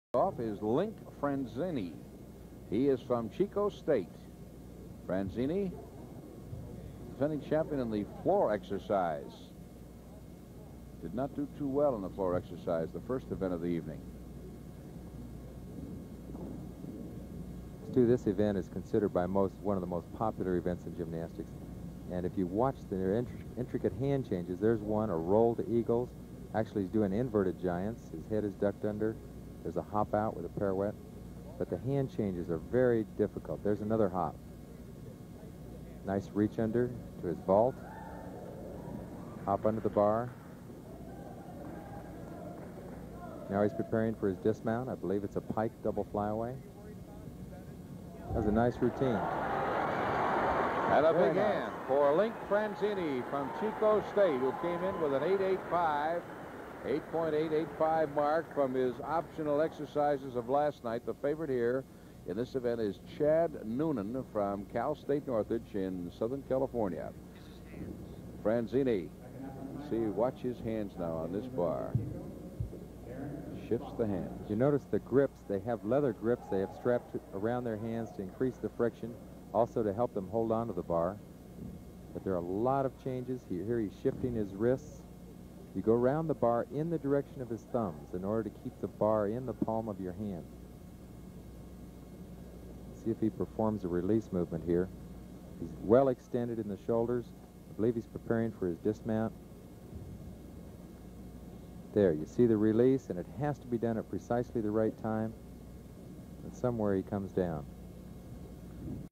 Link Franzini, California State University-Chico, on high bar in finals 1980.
Link Franzini, California State University-Chico, on high bar in finals at the 1980 NCAA Division II Championships at University of California-Davis. Link was the NCAA Division II Champion on floor-ex in 1979 and a five-time All-American in all. Link was a three-time Far Western Conference all-around champion. After graduation Link became a member of the USA Gymnastics national team. Later Link also competed in Tae Kwon Do and bodybuilding at the national level; and he later coached at the Central California Gymnastics Institute in Visalia, CA. Link is a member of the Chico State Athletic Hall of Fame. Link went to Skyline High School in Oakland, California (video courtesy of Ron C Bell).